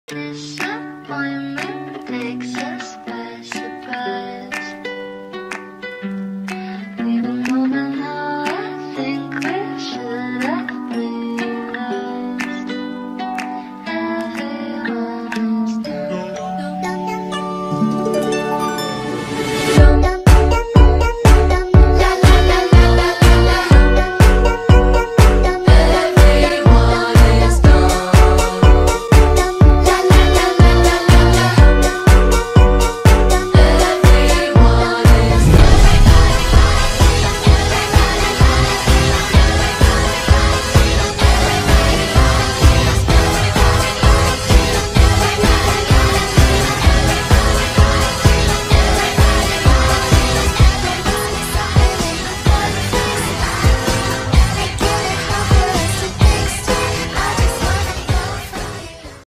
!!FLASHING LIGHTS, GUN, BLOOD!! everyone is dumb || animation meme
ignore any editing mistakes pls my program kept crashing asdjkhasdk

silly bsd ocs make me go heeheehoohoohaha

~ info ~

Total time took: 3 days

Characters used: Ivy, Coda (bsd ocs!)

Programs used: Clipstudiopaint ex, Sony vegas 16

ok bye